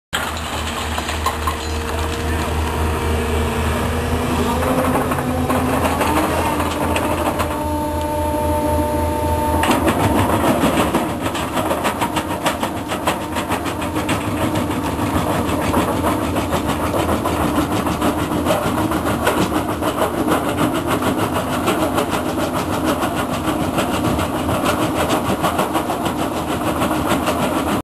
One man operation
Machine can load, crush and stockpile
within a small area.